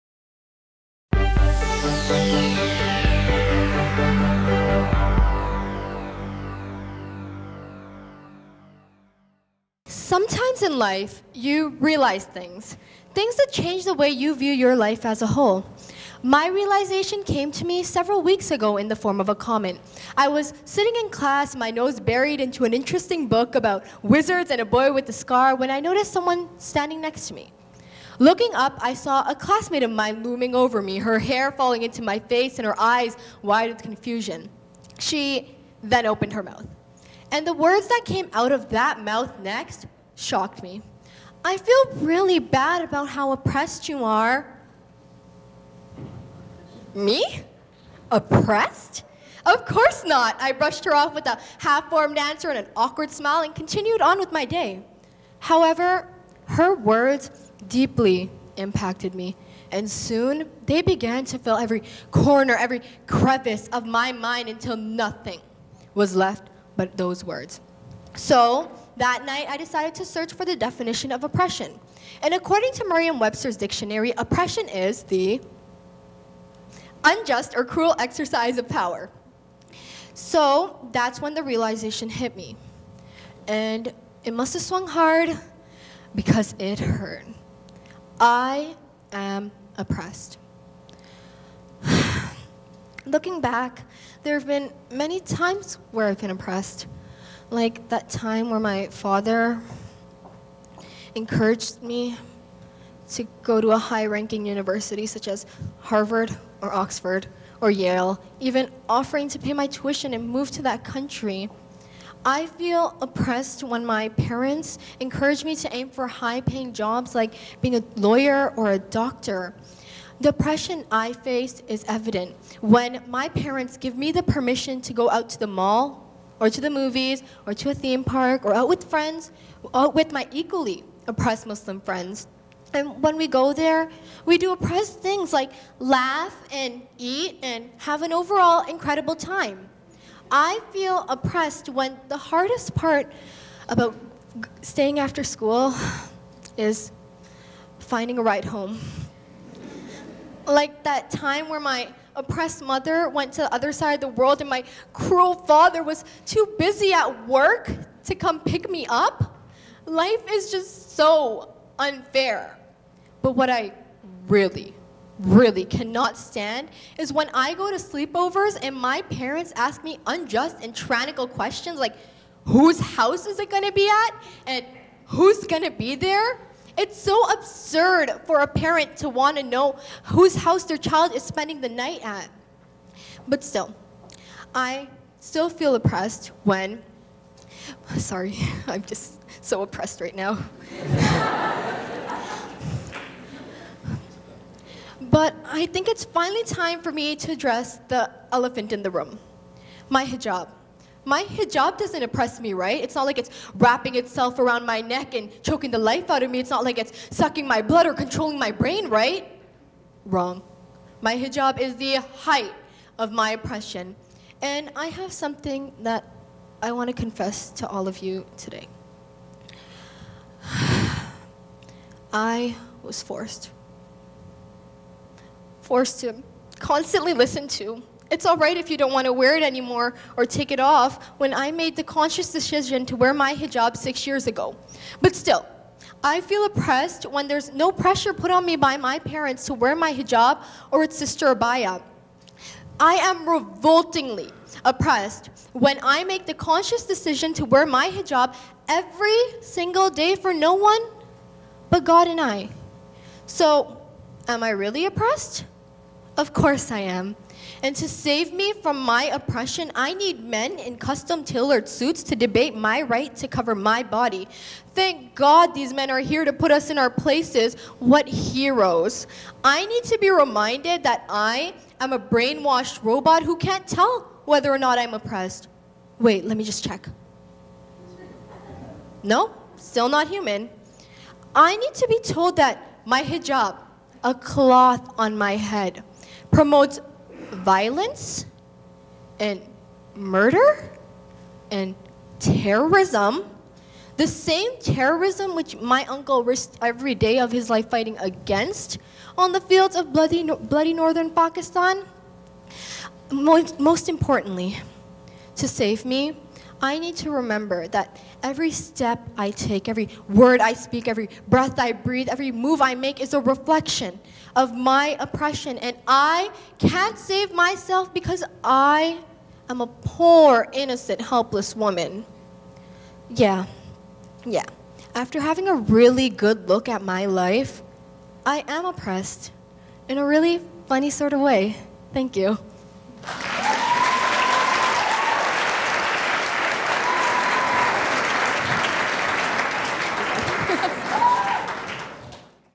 Oppressed? | Samar Moghal | TEDxYouth@BOSS
Society traps us in boxes. These boxes are based on colour, creed, sex, age, and many other social constructions. However, when we do not fit, we struggle to get out of them. This speech explores what it means to be a Muslim woman in the West through the lens of a teenage girl. Through sarcastic language and bittersweet truths, this talk encourages the audience to consider how we can view Muslim women in a different light.

A grade 11 student at Bur Oak Secondary School in Markham, Ontario with plans to become a lawyer and eventually start her own law firm.